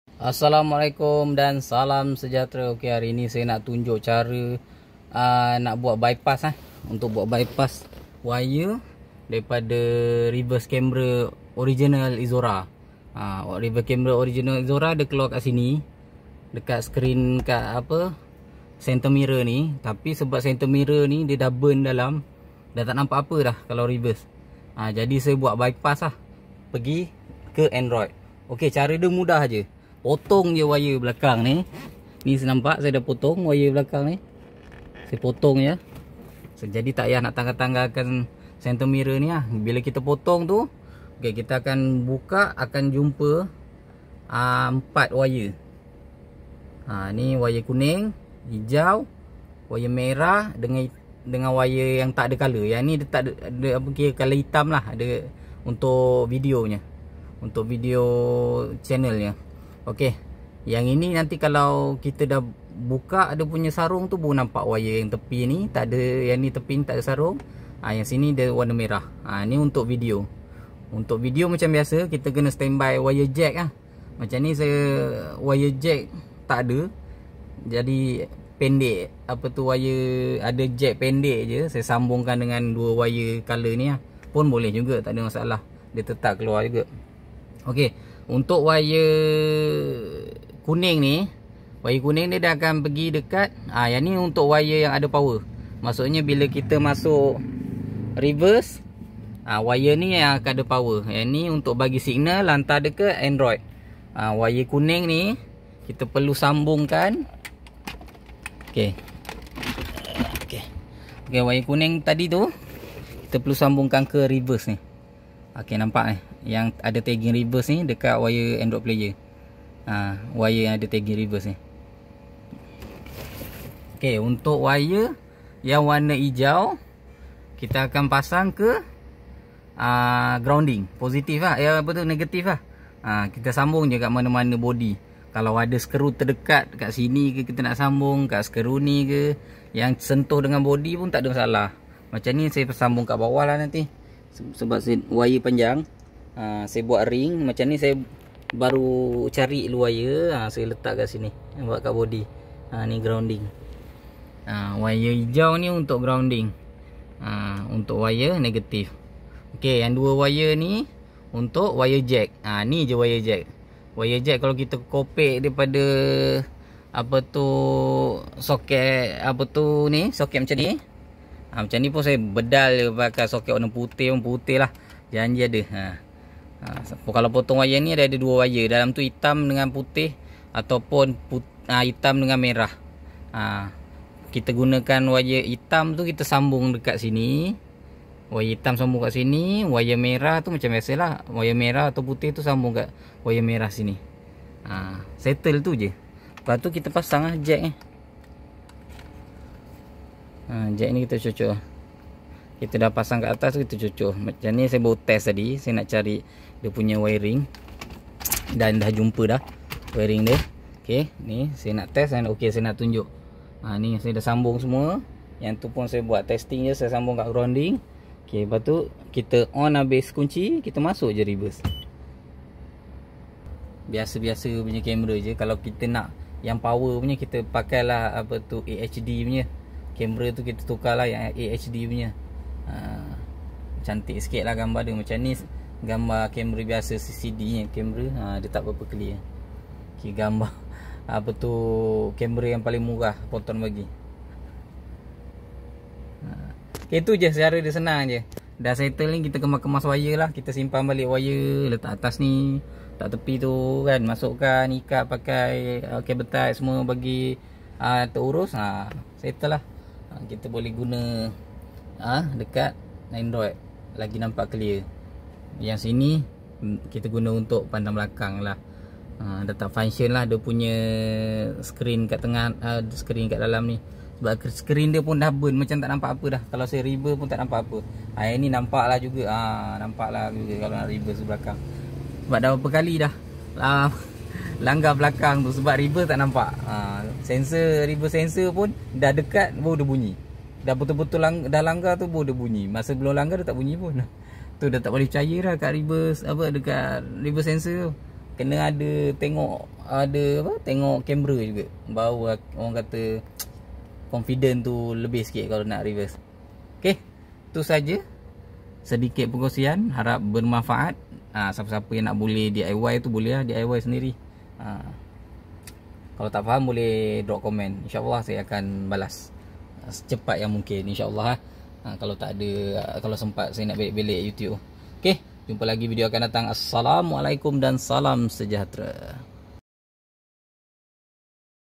Cara Alih Wayar Reverse Camera Asal Proton Exora Ke Android Player
Bismillah... Assalamualaikum dan Salam Sejahtera...

Video kali ini sy nak tunjuk cara alih wayar reverse camera asal proton exora yang kat center mirror tu ke android player...

Sebab alihnya screen dekat center mirror tu dah burn tak nampak apa dan terlalu kecil untuk tengok kalau nak reverse...

Alih dekat android player lebih besar dan senang nak tengok...